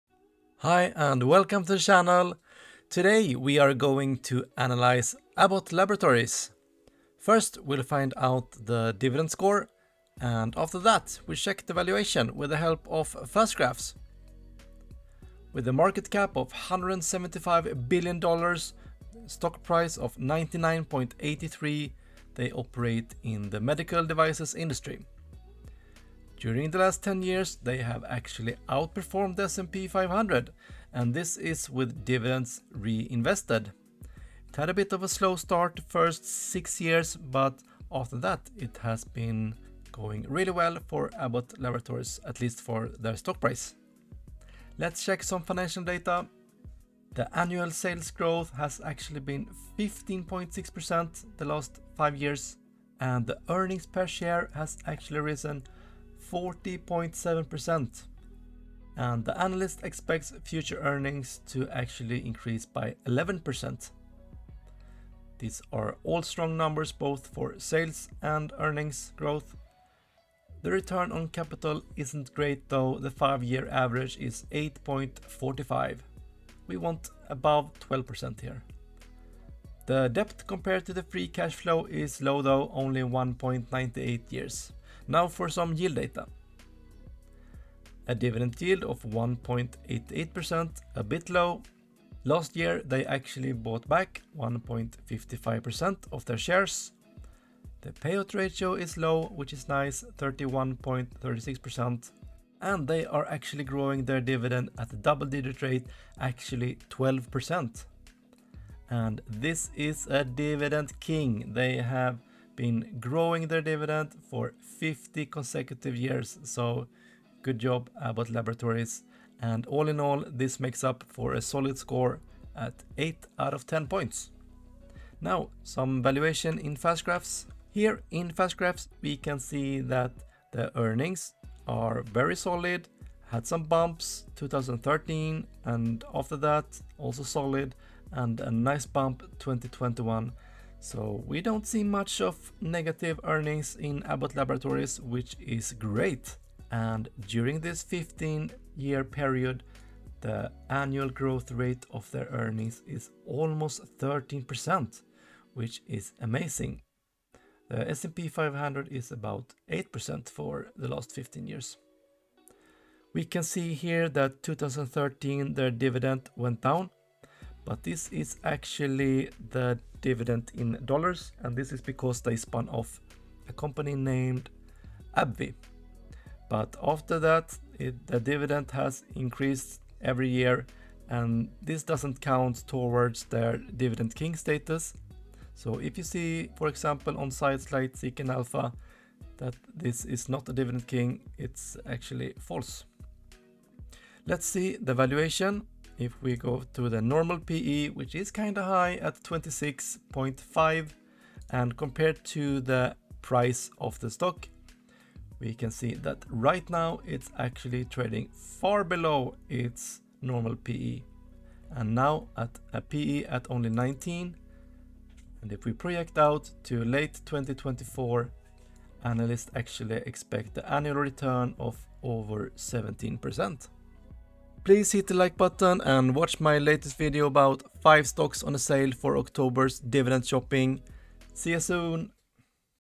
Abbott Laboratories (ABT) - Dividend Stock Analysis
What dividend score does Abbott Laboratories get? Is the stock undervalued or overvalued? Find out in this clear-cut analysis!

✅ Thresholds ✅
Sales Past (5Y): Over 4%
EPS Past (5Y): Over 8%
EPS  Next (5Y): Over 8%
ROIC Past (5Y): Over 12%
Debt/FCF: Under 5 years

Dividend Yield: 2-6%
Buyback Yield: Positive
Payout Ratio: Under 70%
Growth Past (5Y): Over 5%
Growth Years: Over 10 years